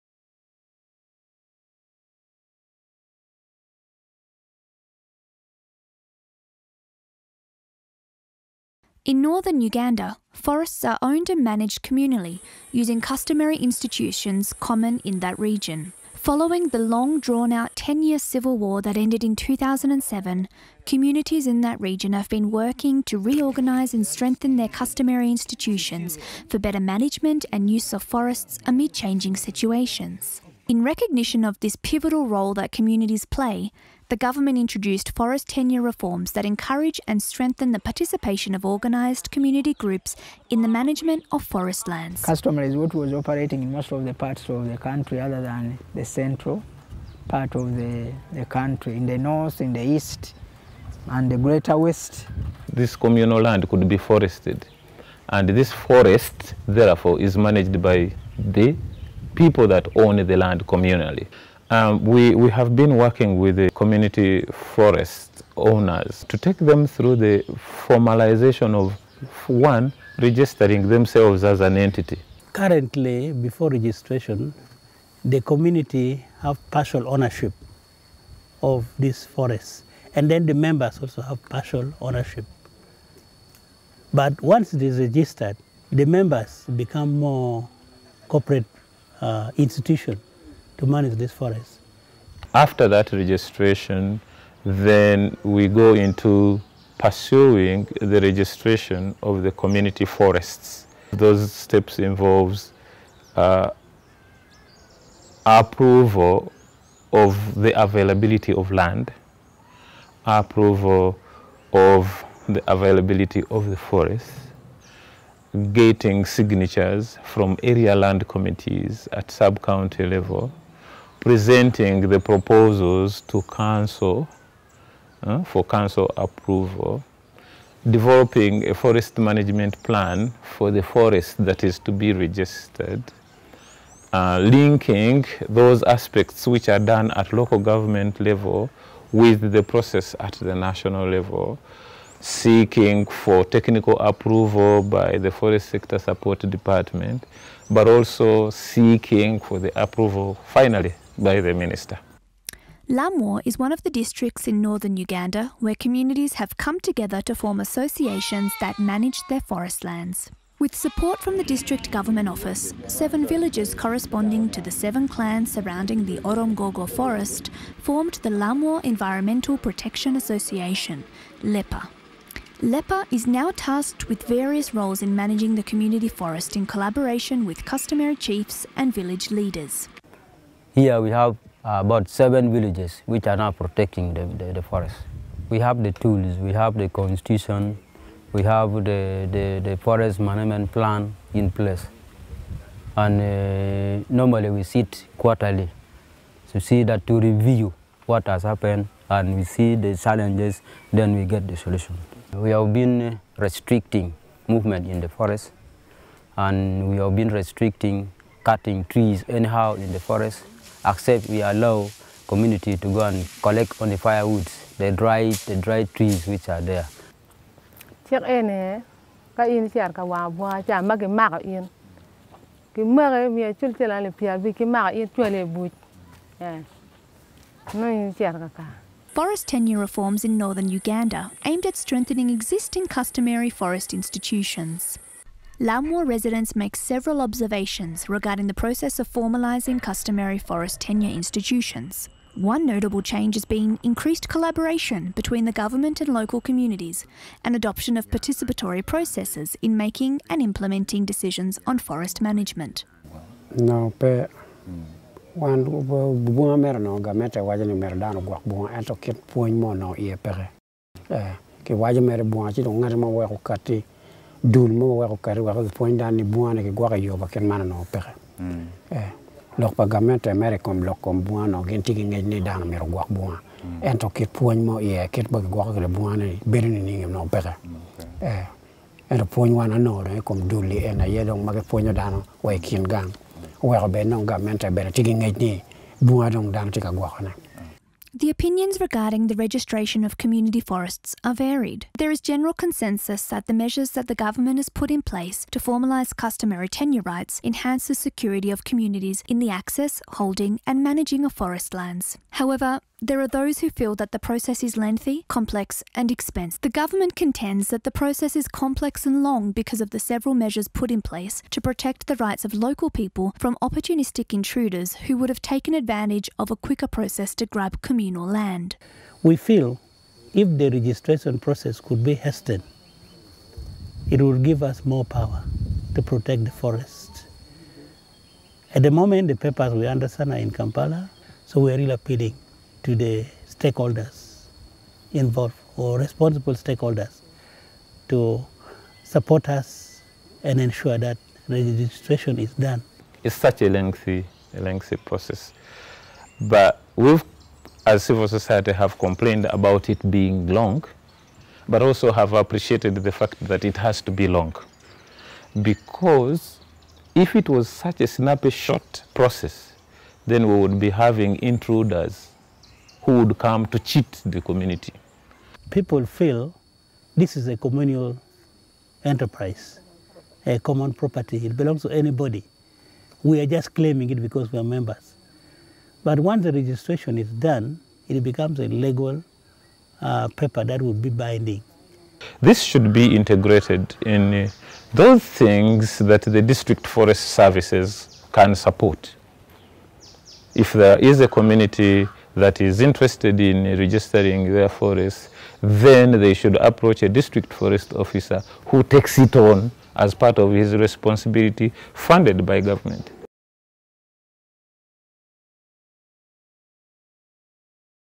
Customary rights and forest tenure reform in Uganda
In northern Uganda, forests are owned and managed communally by customary institutions. Since the 10-year civil war in the area ended in 2007, communities have been working to reorganize those institutions, and the national government has introduced tenure reforms to strengthen the participation of community groups in managing forest land. In this video, stakeholders in northern Uganda provide insights into the process of forest reform, and community representatives recount their experiences of trying to formalize customary forest rights. The video is part of a series on the implementation of forest tenure reform in Uganda, which emerged from the Global Comparative Study on Land Tenure Reform, led by the Center for International Forestry Research (CIFOR).